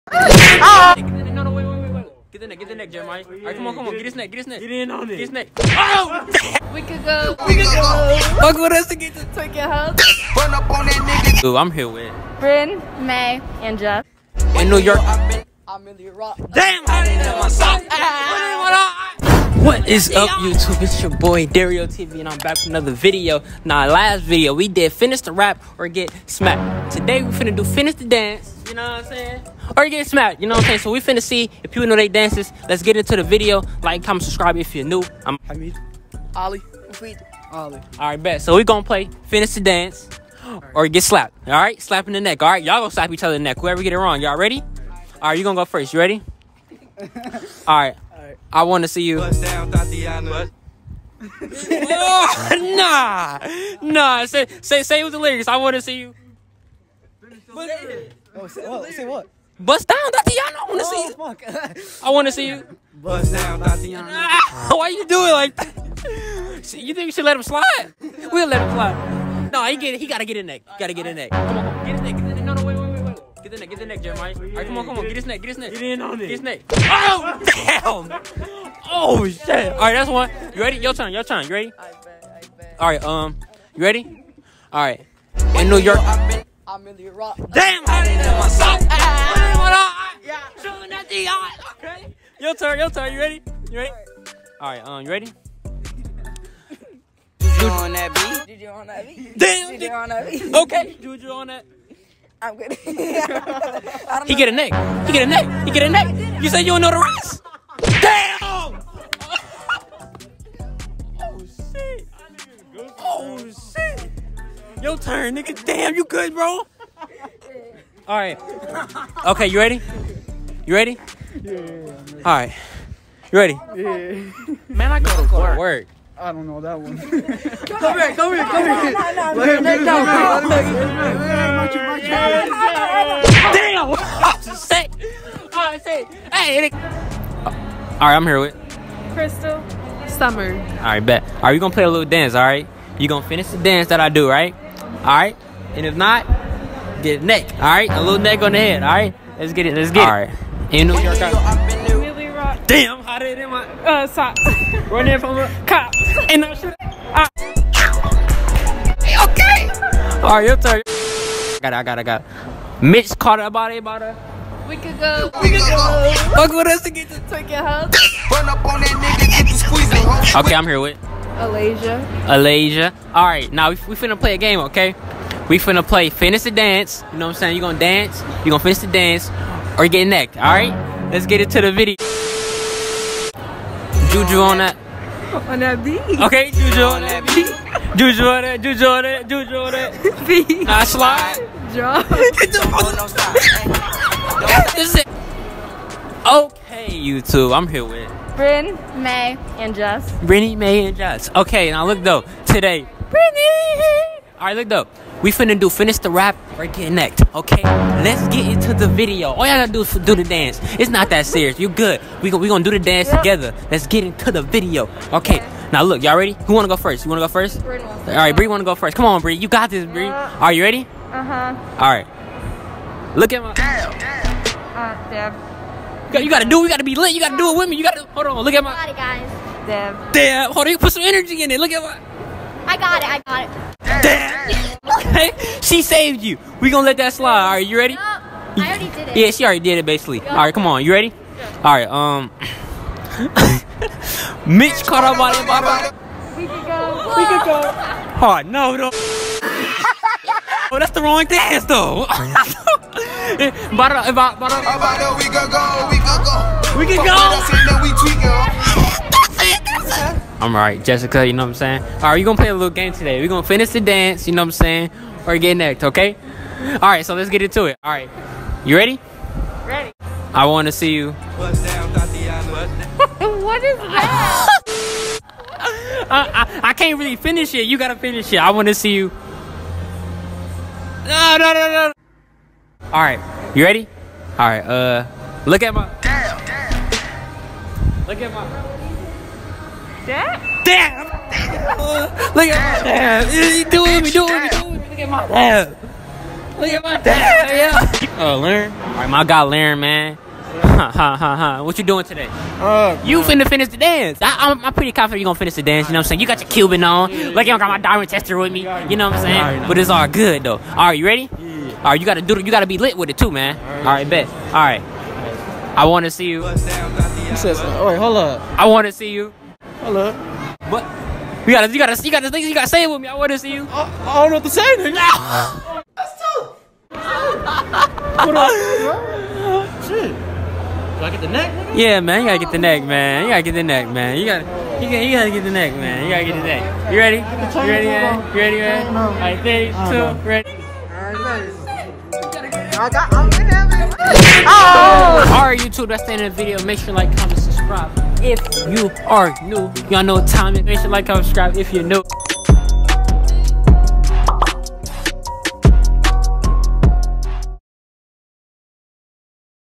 Finish The Dance Or Get Punished 🤕🕺Atlanta Mall Edition | Public Interview
IM FINNA BLOW UP MAN LETS GOO GRIND DONT STOP